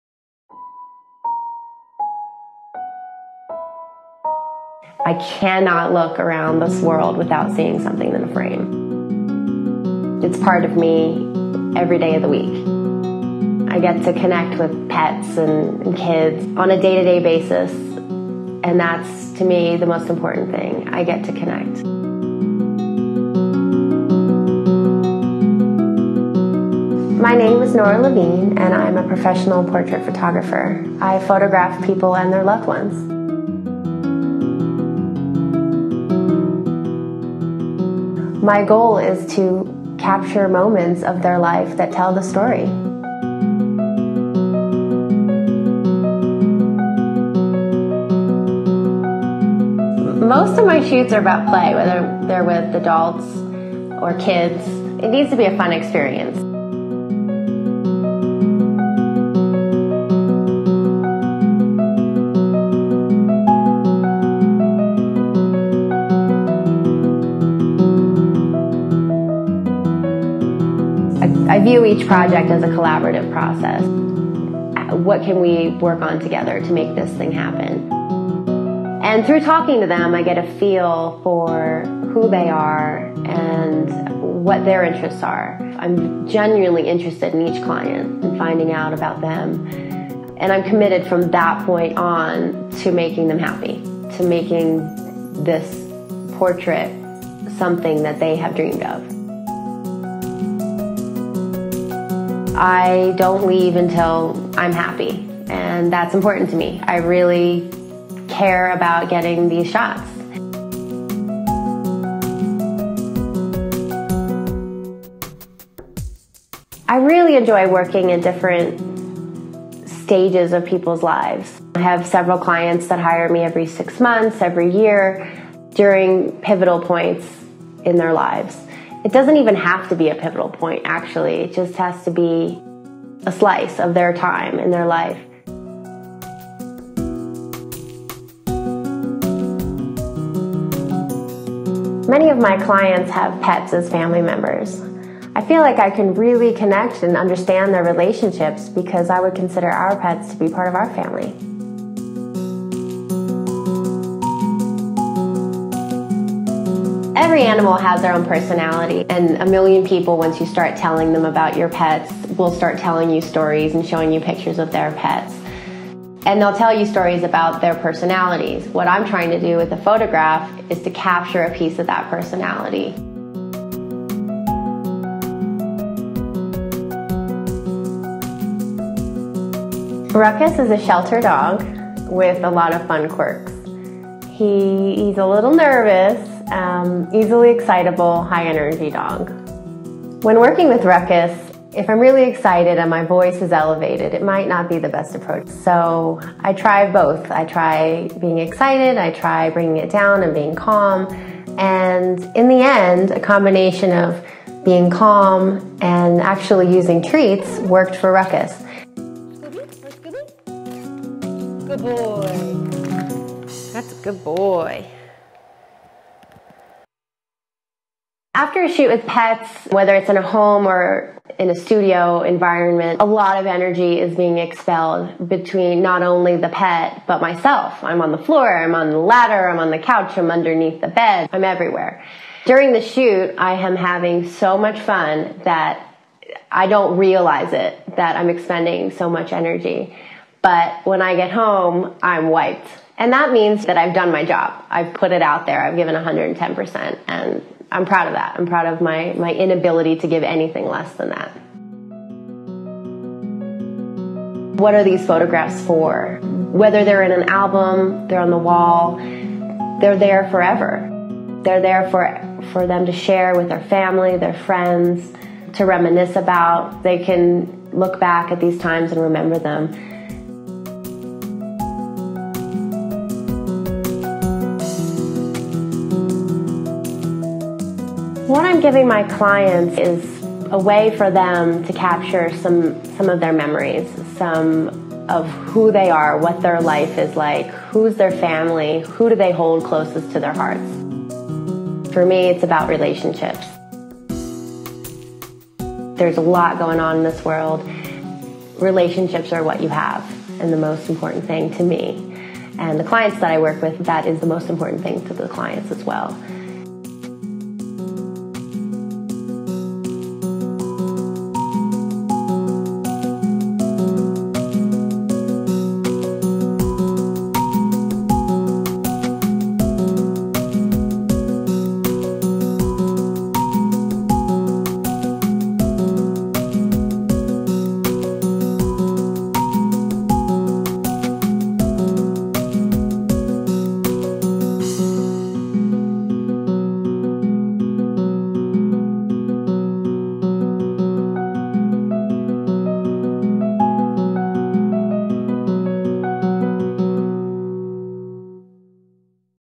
Norah Levine Photography Austin, TX and Santa Fe, NM
A "behind the lens" interview of Austin Texas and Santa Fe New Mexico portrait photographer, Norah Levine. Norah specializes in creative imagery of children and pets on location.